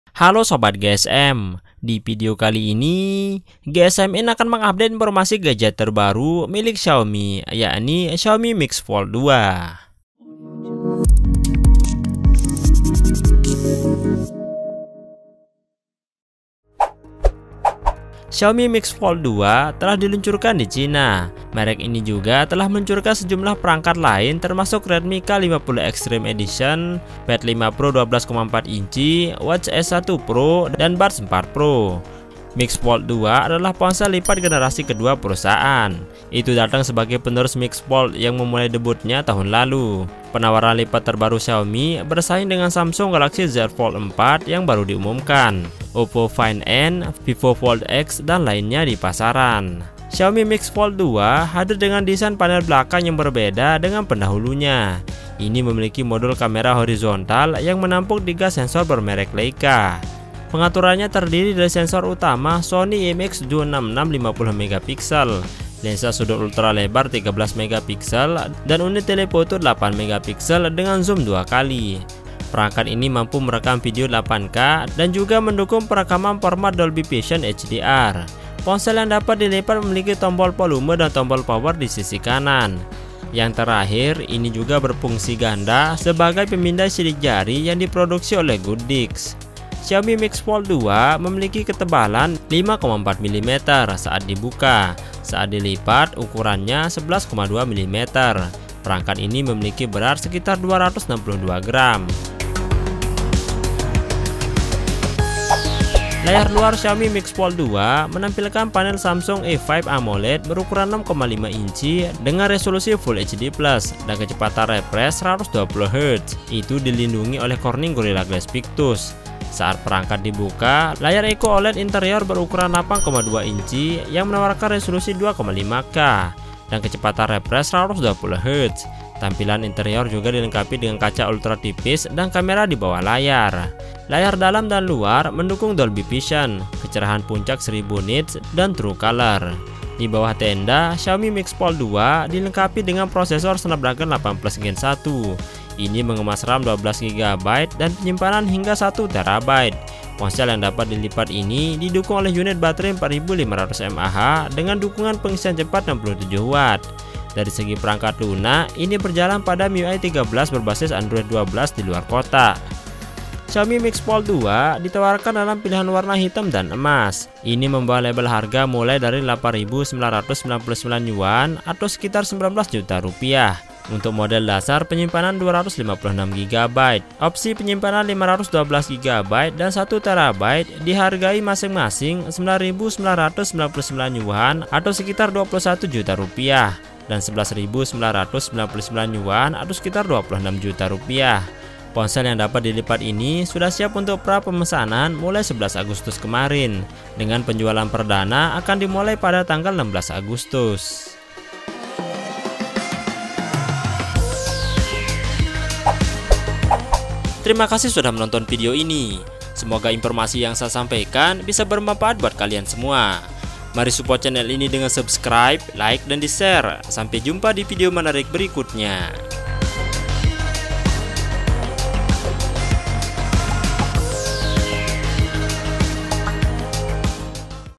Lebih Murah dari Galaxy Z Fold 4! XIAOMI MIX FOLD 2 Resmi Rilis! Harga dan Spesifikasi Indonesia!
Xiaomi MIX Fold 2 telah diluncurkan di China. Merek ini juga telah meluncurkan sejumlah perangkat lain termasuk Redmi K50 Extreme Edition, Pad 5 Pro 12.4, Watch S1 Pro, dan Buds 4 Pro. Xiaomi Mix Fold 2 adalah ponsel lipat generasi kedua perusahaan. Itu datang sebagai pesaing Samsung Galaxy Z Fold 4 yang baru diumumkan, OPPO Find N, Vivo Fold X, dan lainnya di pasaran.